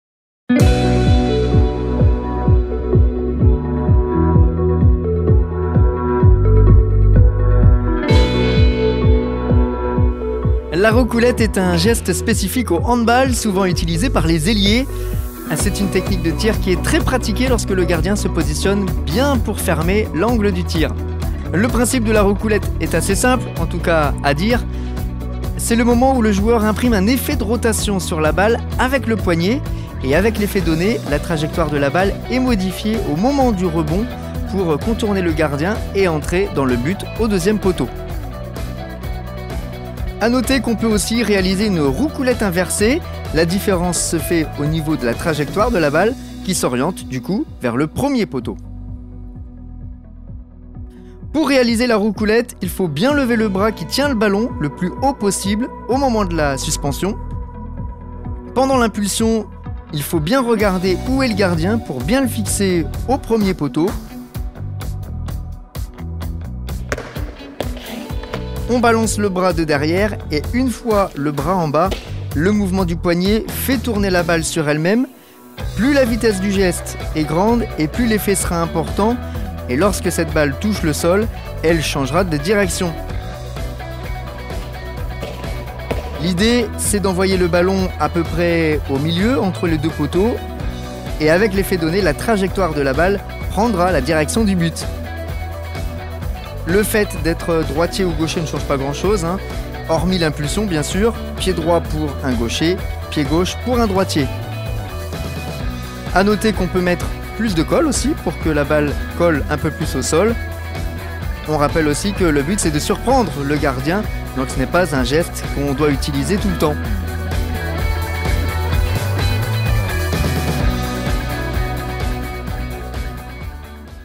COMMENT RÉALISER UNE ROUCOULETTE AU HANDBALL / Geste Technique #3
➡ Chaque mois, Julien détaille un geste propre à une discipline. Pour ce nouveau numéro, zoom sur la roucoulette, geste propre au handball, grâce à Foudhil Haddad, de l'AS St-Brice Courcelles Handball.